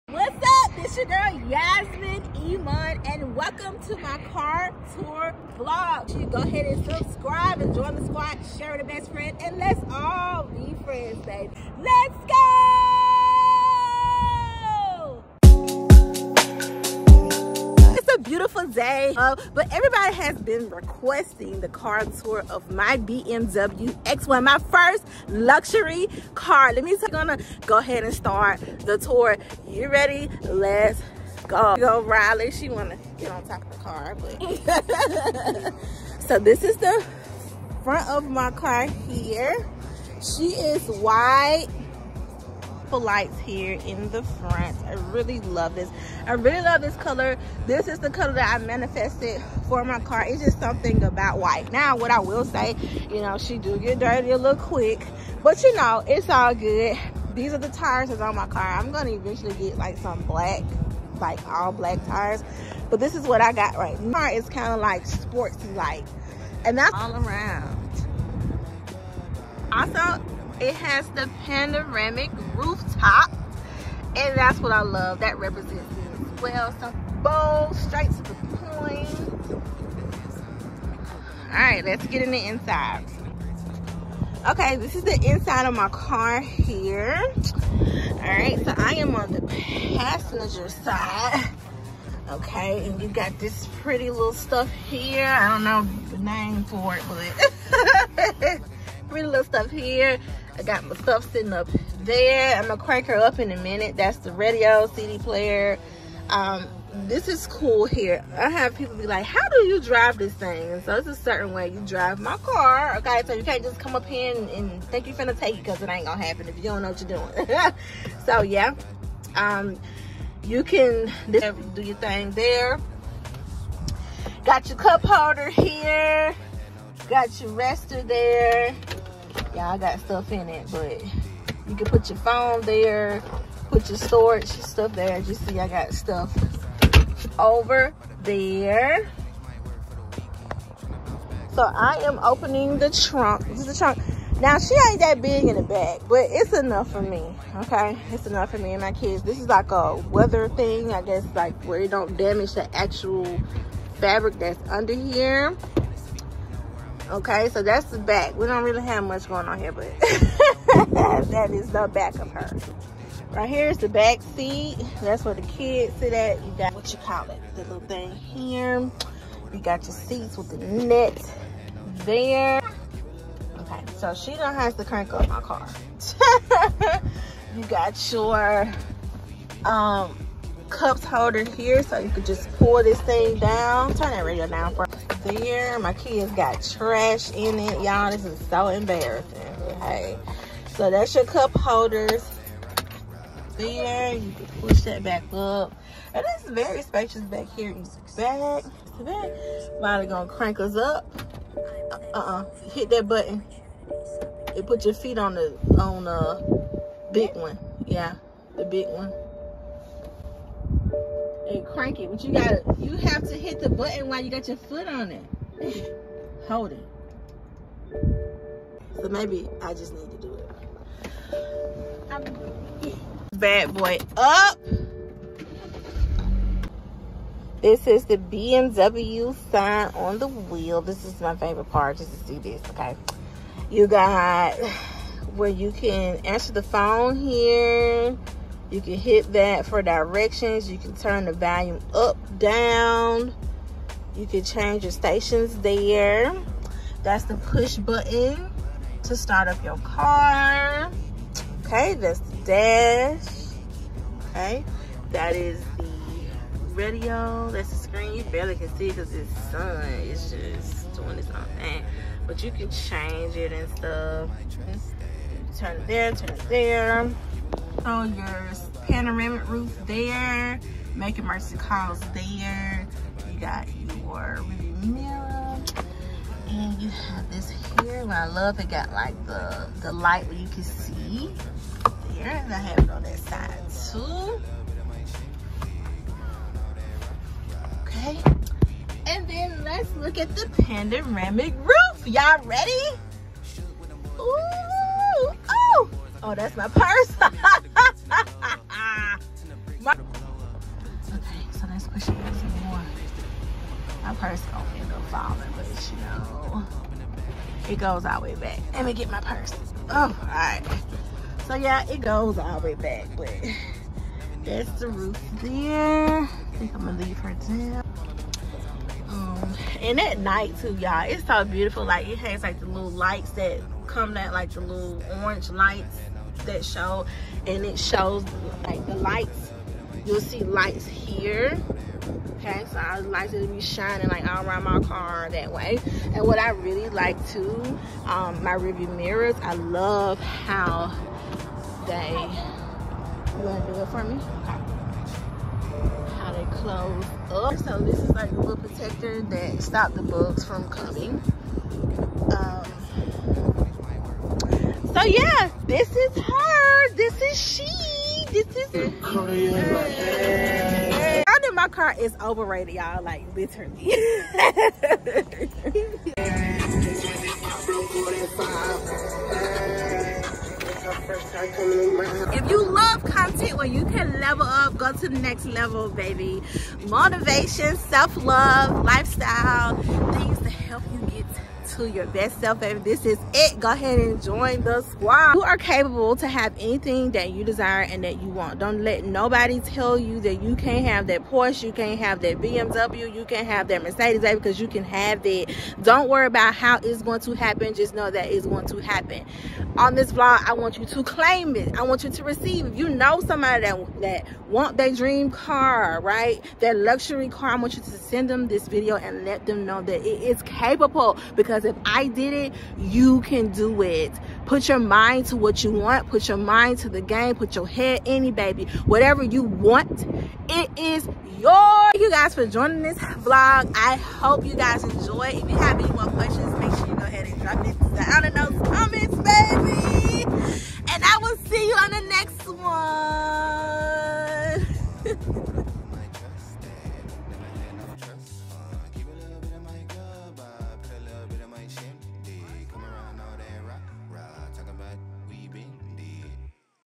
New Car Tour 2022!!|My first luxury car| BMWX1
Heyyy Besties!!

I’m super excited about this video, today I will be taking you all on a tour of my first luxury car that I manifested and was blessed with a few months ago. Let’s go on this journey together.☺️
This is some inspiration to show you that you can have anything you desire.